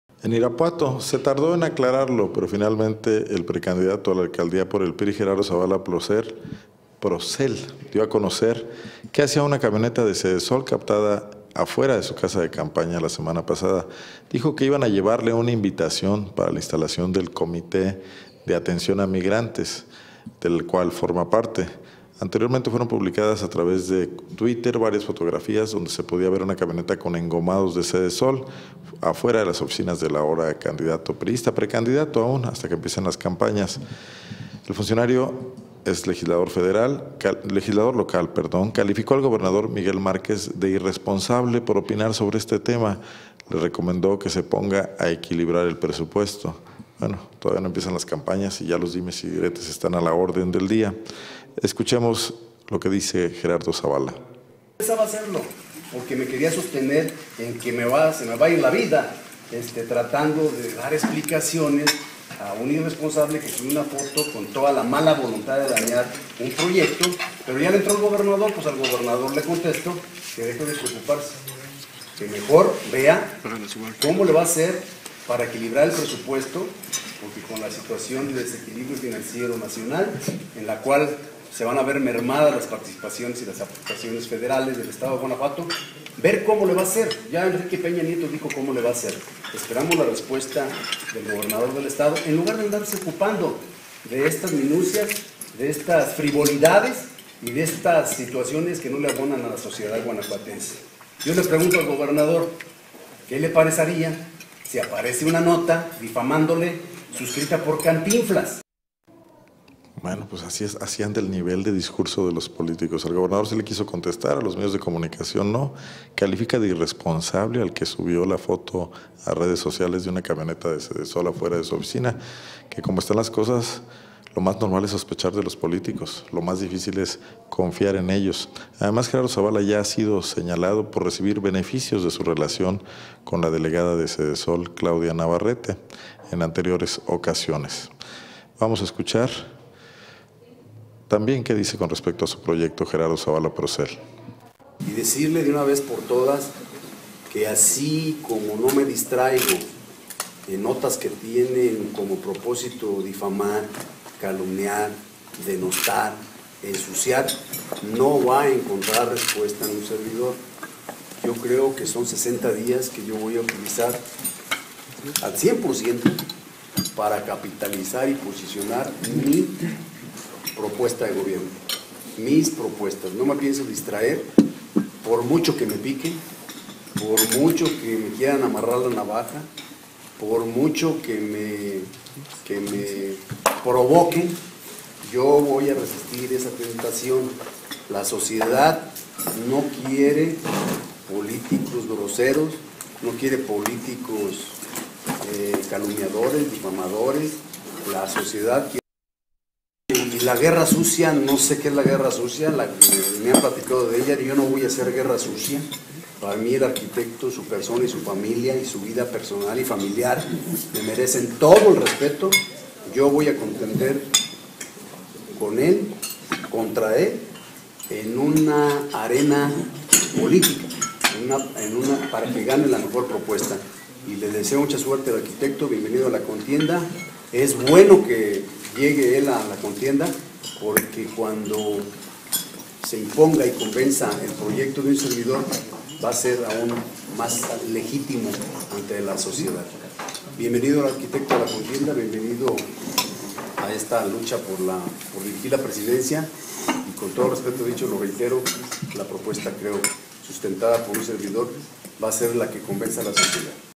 Justifica Gerardo Zavala presencia de camioneta de Sedesol afuera de su oficina
Gerardo Zavala Procell, candidato la alcaldía de Irapuato por el PRI,
aclaró que la camioneta de Sedesol que fue captada afuera de su casa de campaña acudió a llevarle una invitación para la instalación del Comité de Validación y Atención a Migrantes (Covam).